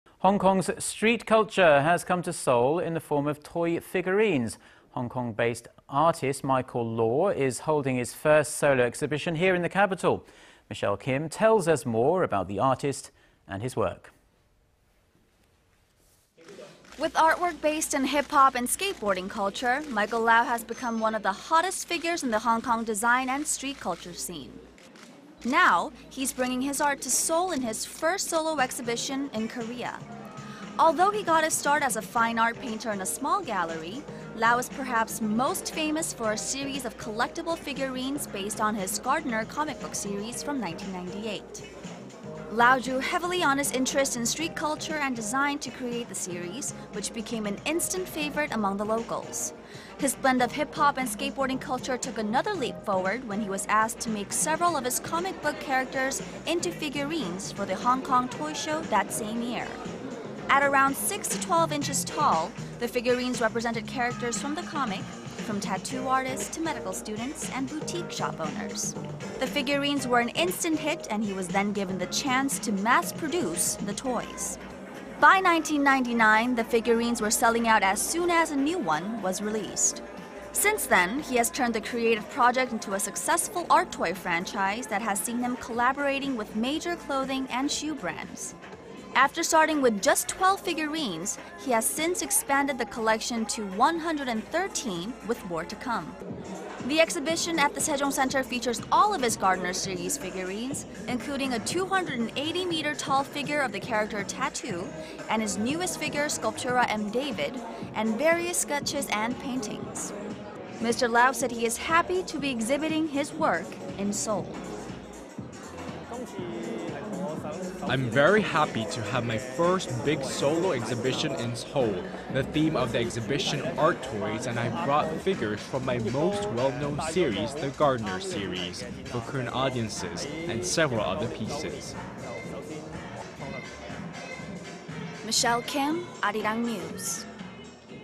Michael Lau Exhibition in Seoul 마이클라우 전시회
Hong Kong's street culture has come to Seoul in the form of toy figurines.

Hong Kong-based artist Michael Lau is holding his first solo exhibition here in the capital.

Michelle Kim tells us more about the artist and his work. With artwork based in hip-hop and skateboarding culture, Michael Lau has become one of the hottest figures in the Hong Kong design and street culture scene.

Now, he's bringing his art to Seoul in his first solo exhibition in Korea.

Although he got his start as a fine art painter in a small gallery, Lau is perhaps most famous for a series of collectable figurines based on his "Gardener" comic book series from 1998.

Lau drew heavily on his interest in street culture and design to create the series, which became an instant favorite among the locals.

His blend of hip-hop and skateboarding culture took another leap forward when he was asked to make several of his comic book characters into figurines for The Hong Kong Toy Show that same year.

At around 6 to 12 inches tall, the figurines represented characters from the comic, from tattoo artists to medical students and boutique shop owners.

The figurines were an instant hit and he was then given the chance to mass produce the toys.

By 1999, the figurines were selling out as soon as a new one was released.

Since then, he has turned the creative project into a successful art toy franchise that has seen him collaborating with major clothing and shoe brands.

After starting with just 12 figurines, he has since expanded the collection to 113 with more to come.

The exhibition at the Sejong Center features all of his "Gardner" series figurines including a two-hundred-eighty-meter tall figure of the character Tattoo and his newest figure Sculptura M David and various sketches and paintings.

Mr. Lau said he is happy to be exhibition his work in Seoul.

"I'm very happy to have my first big solo exhibition in Seoul. The theme of the exhibition is Art Toys and I've brought figures from my most well-known seires, the 'Gardener' series, for Korean audiences, and several other pieces."